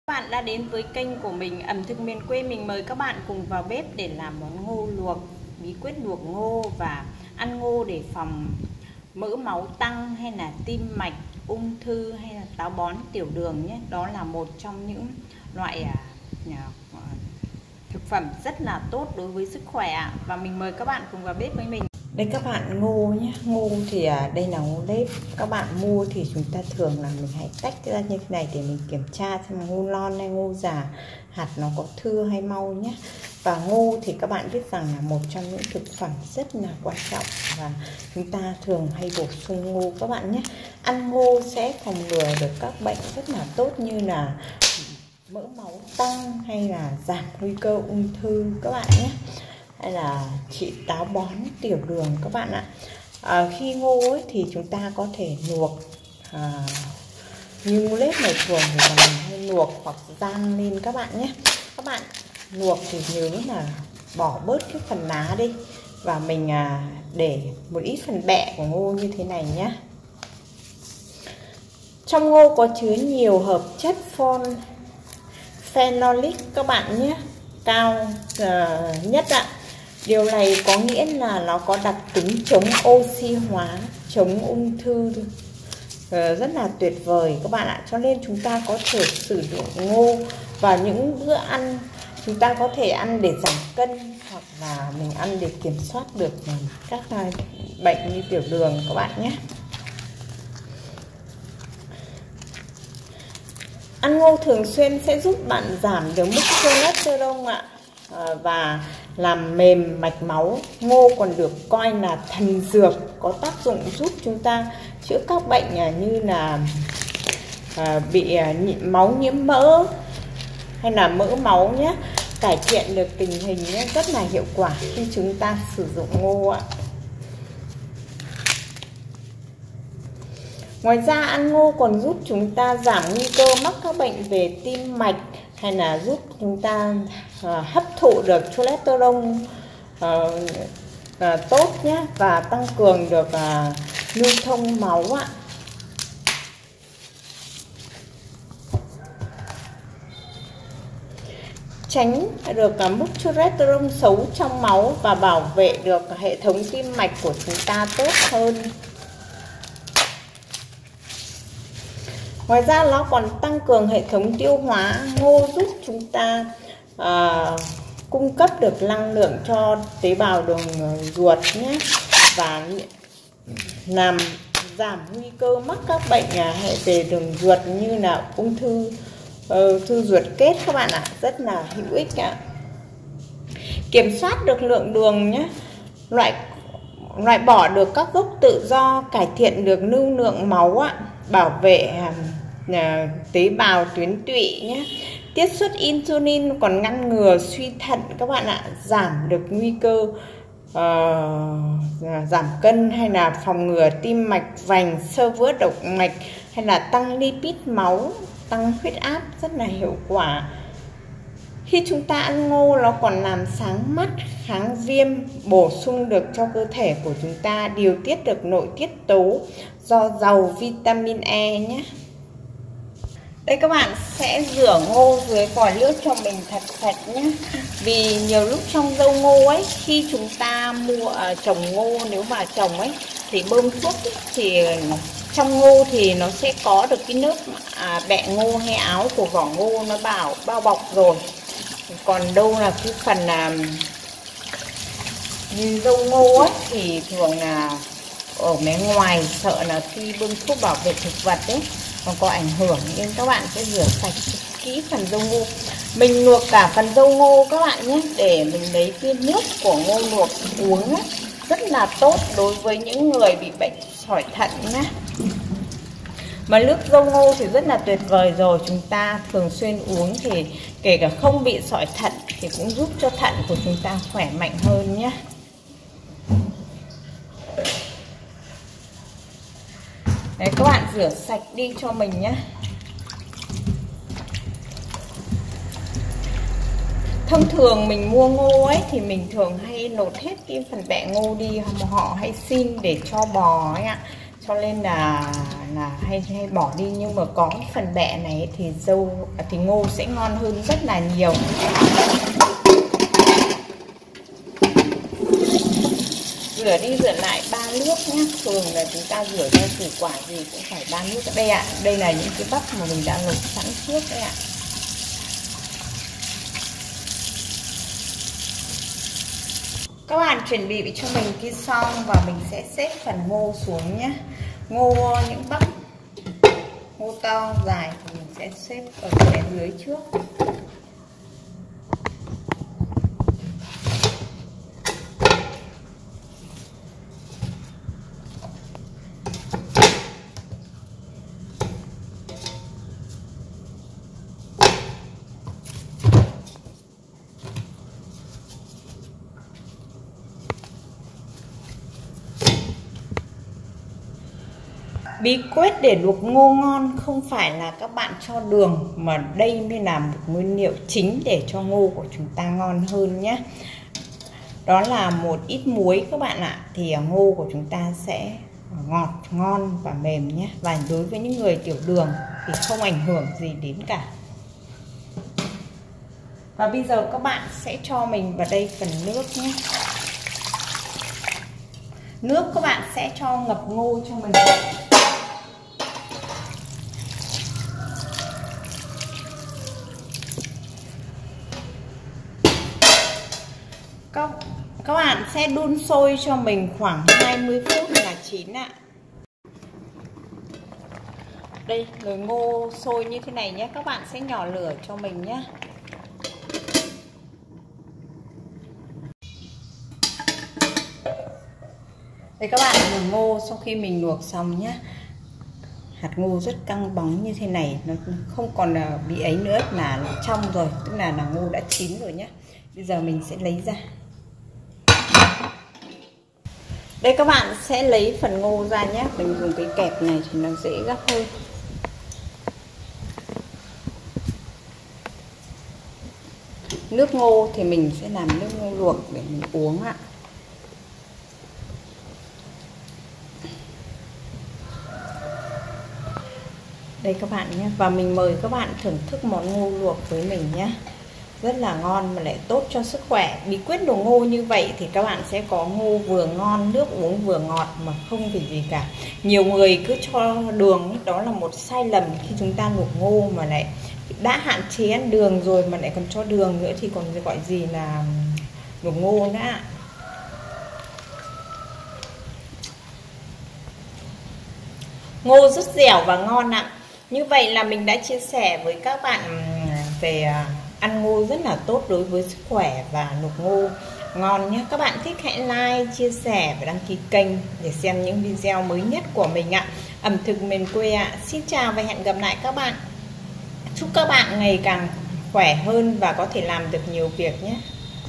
LUỘC NGÔ CHỈ THÊM THÌA NÀY NGÔ MỀM NGON NGỌT_ĂN NGÔ PHÒNG MỠ MÁU TĂNG, TIM MẠCH, UNG THƯ, TIỂU ĐƯỜNG
LUỘC NGÔ CHỈ THÊM THÌA NÀY NGÔ MỀM NGON_ĂN NGÔ PHÒNG MỠ MÁU TĂNG, TIM MẠCH, UNG THƯ, TIỂU ĐƯỜNG.